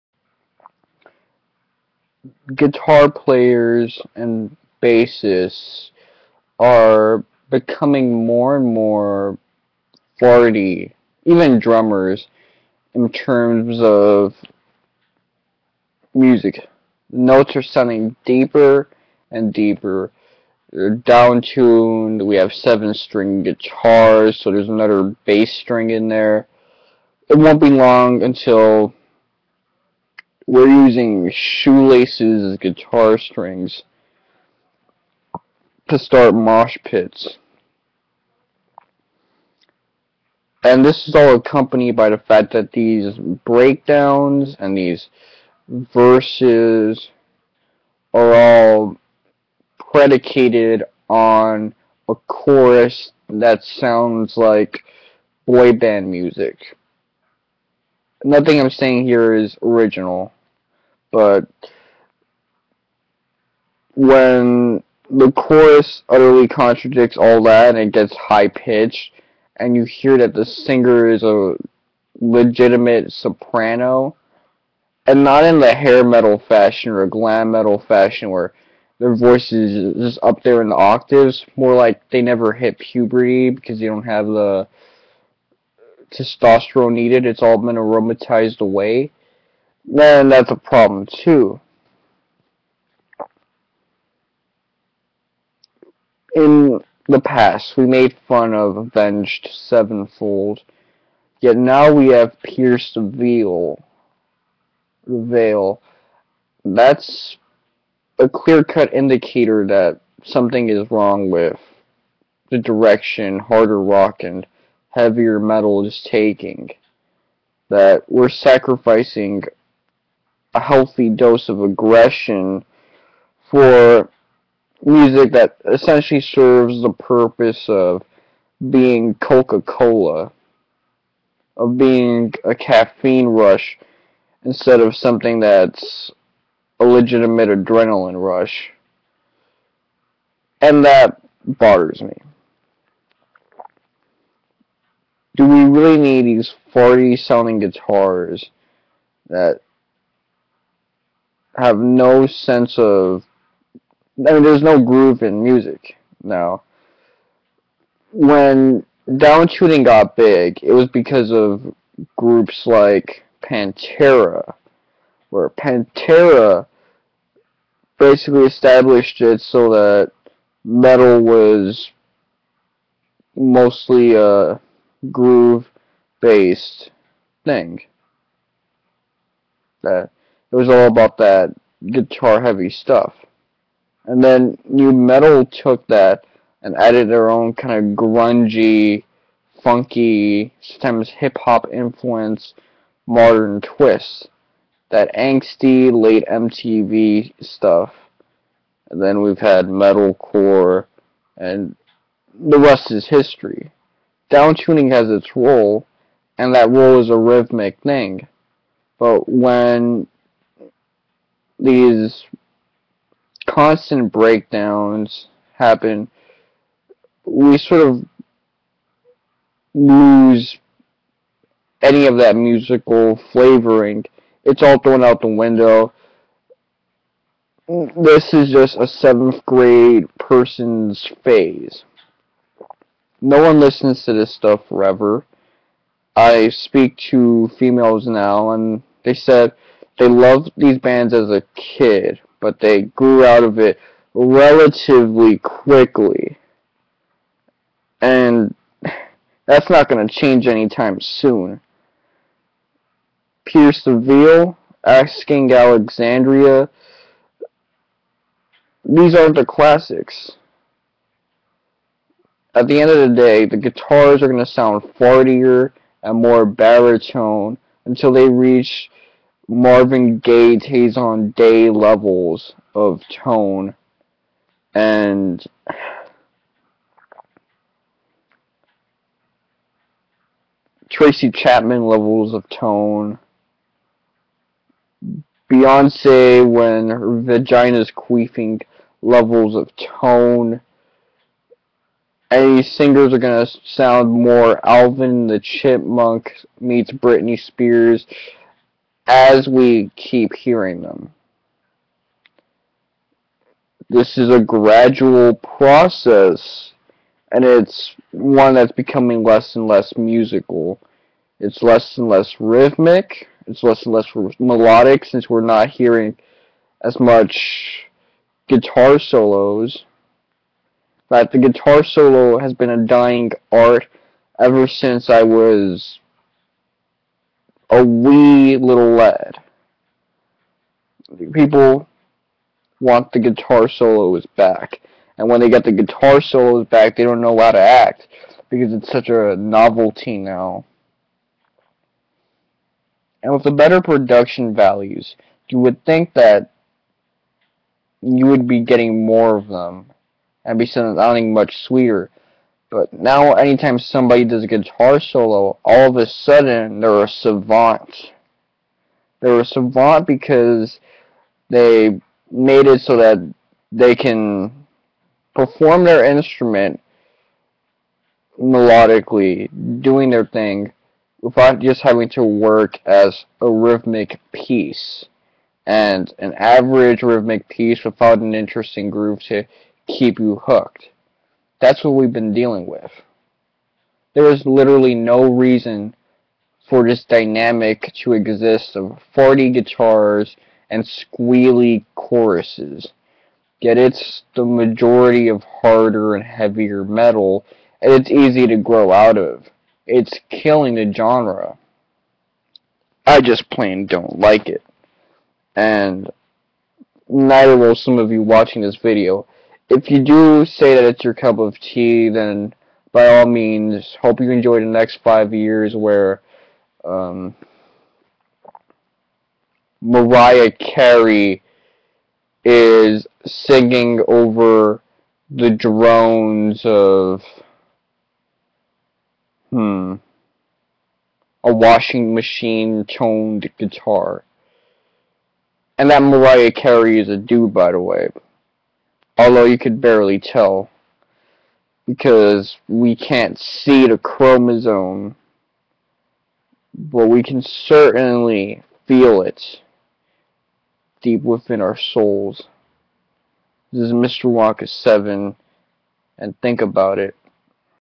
Excessive Downtuning in Metal
A gradual process that I'm not fond of.